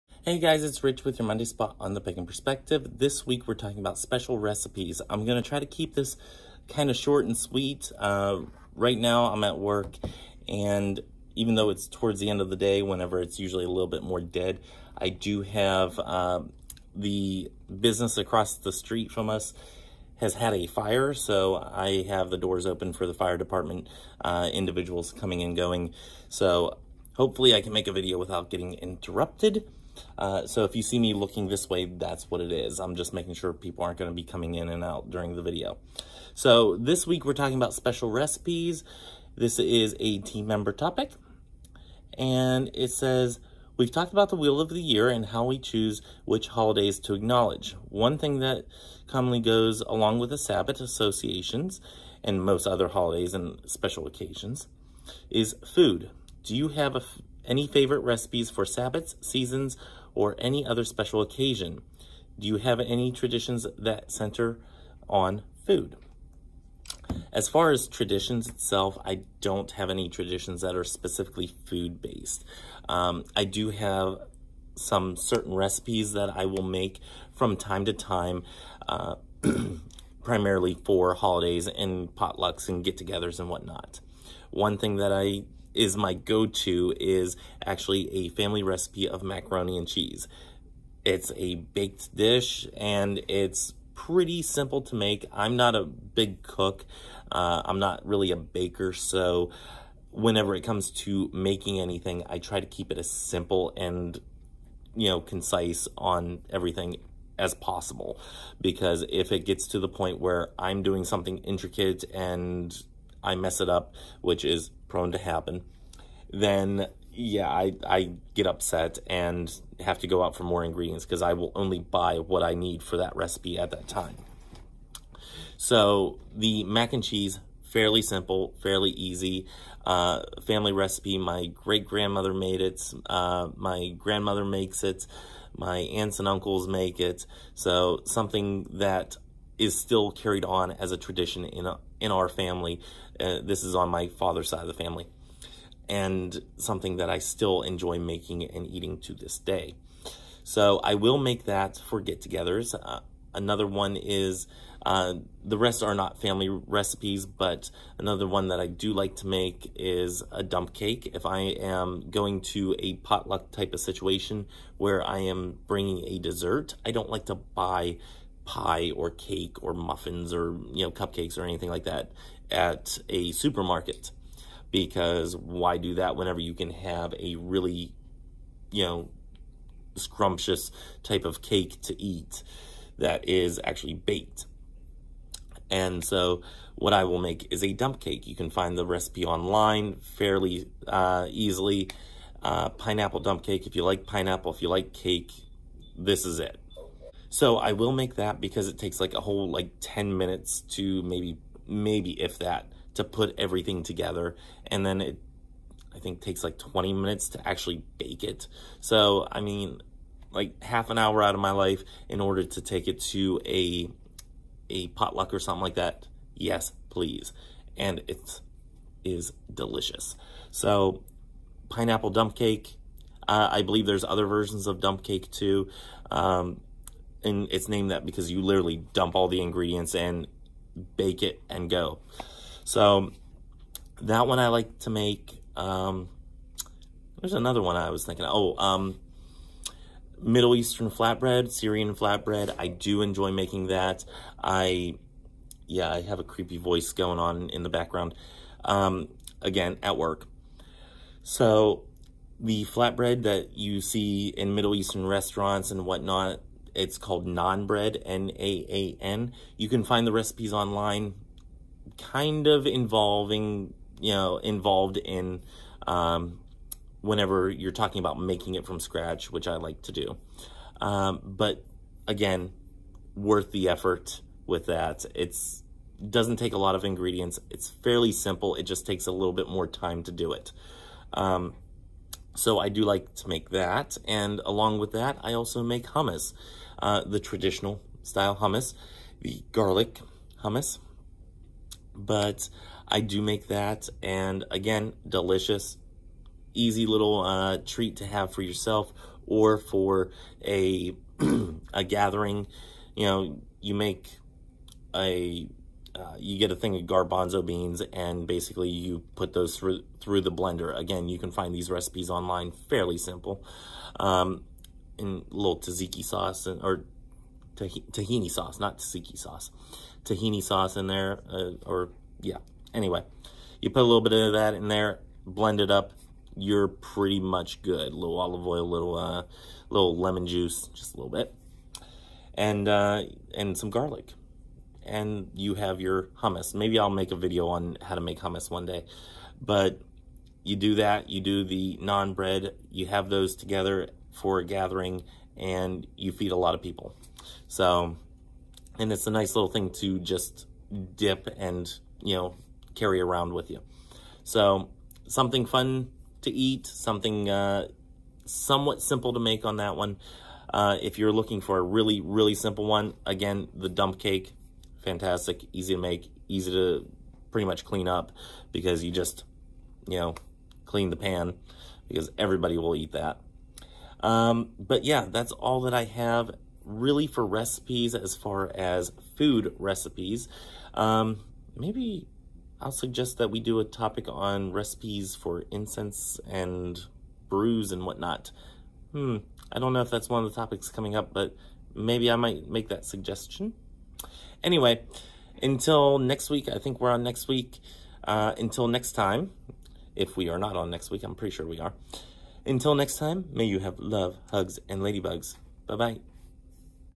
[8.03] Specialty Recipes
This week's topic:
PaganPerspective - Special Recipes
" We've talked about the Wheel of the Year and how we choose which holidays to acknowledge. One thing that commonly goes along with Sabbat associations (and most other holidays and special occasions) is food! Do you have any favorite recipes for sabbats, seasons, or any other special occasion? Do you have any traditions that center on food? "

christowitch2005